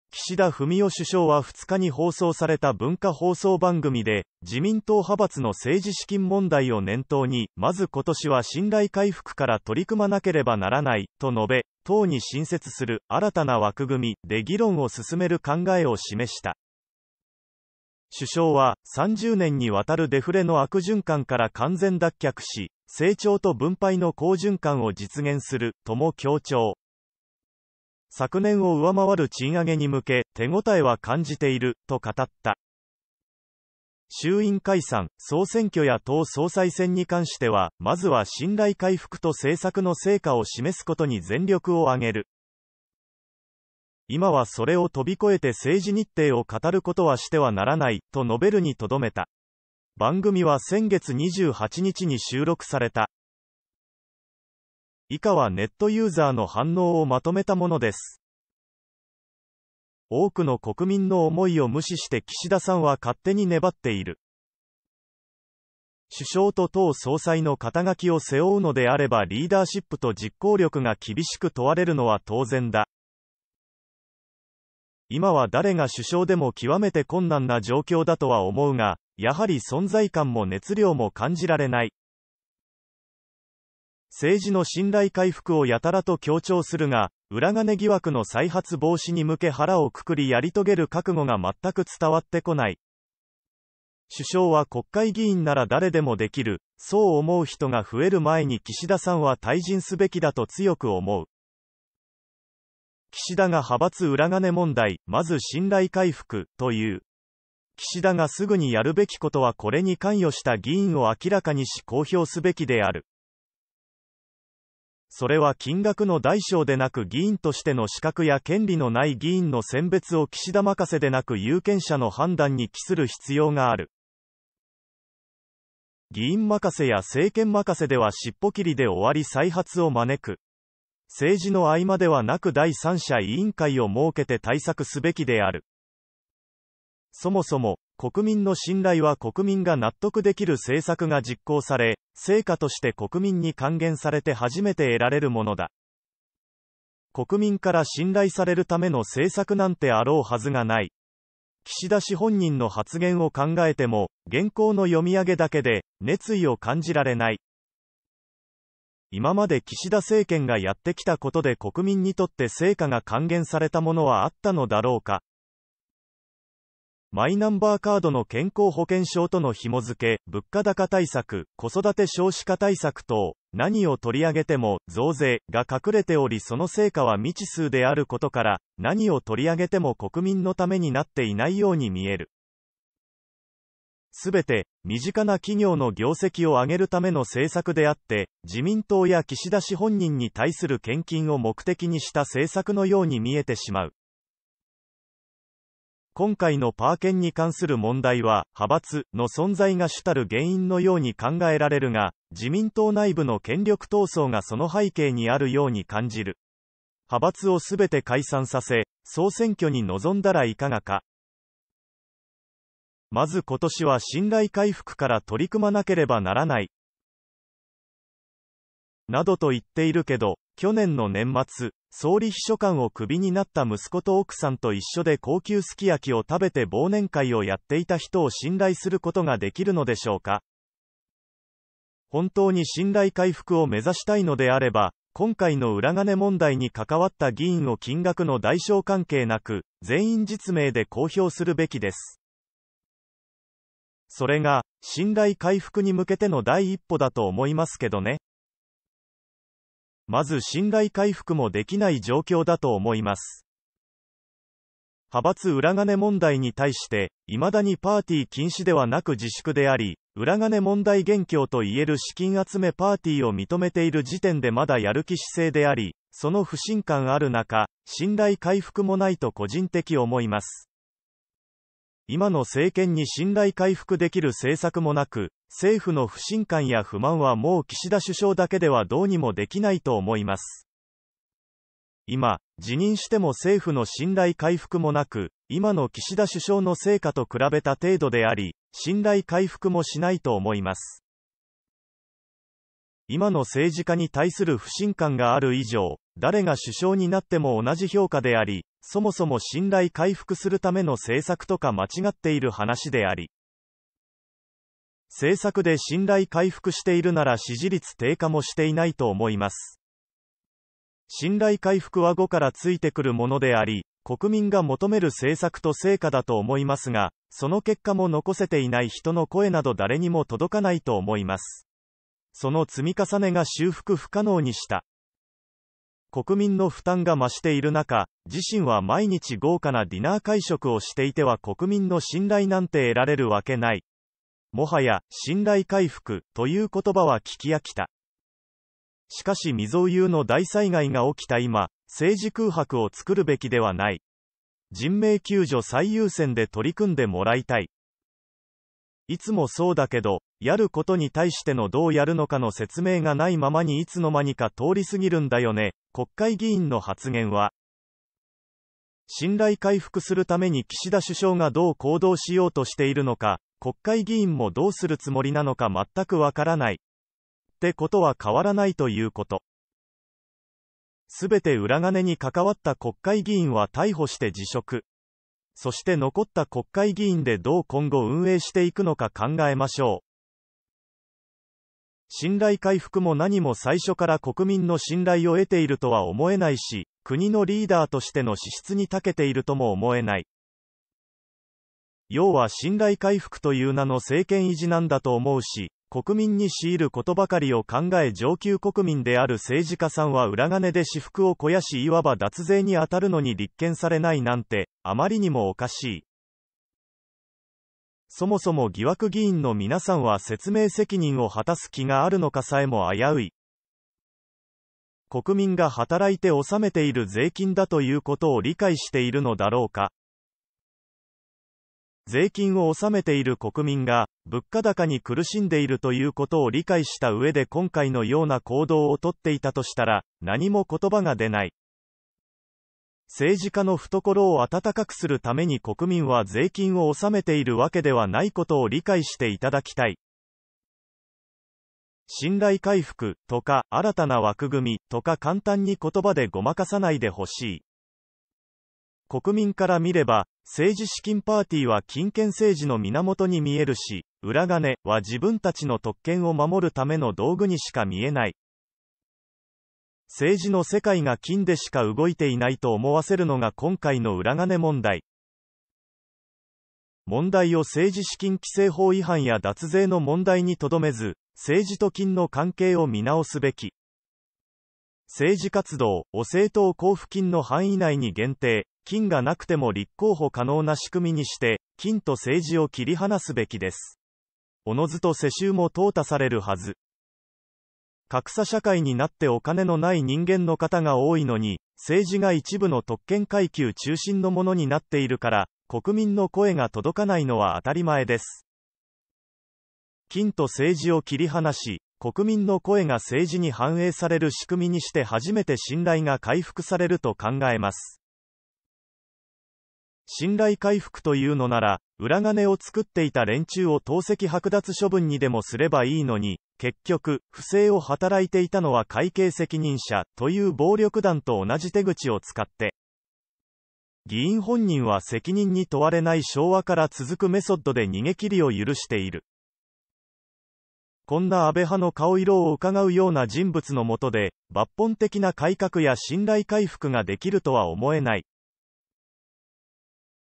岸田首相「まず信頼回復」 派閥裏金問題 岸田首相の信頼回復に対する疑問が多い
政治と金の関係を見直すべきとの意見もある